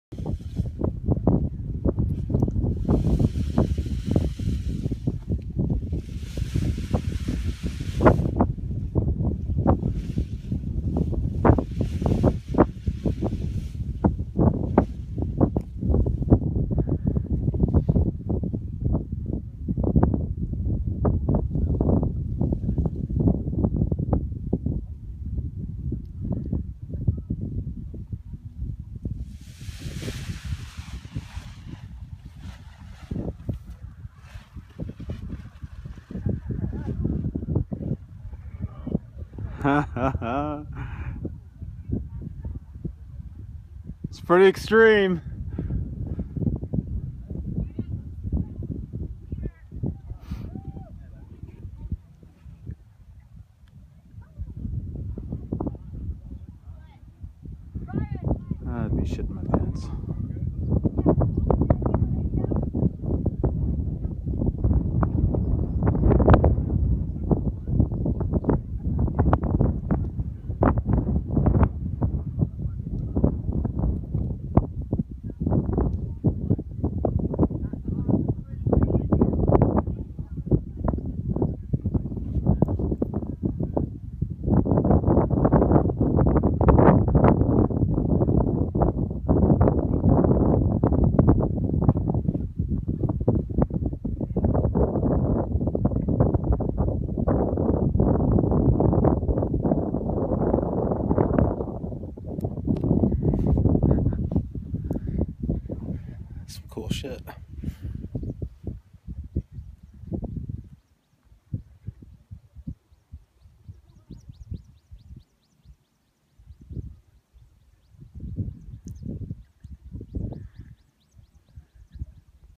Flattop 5/16/15
1st climb of the year and some dude flying like an eagle.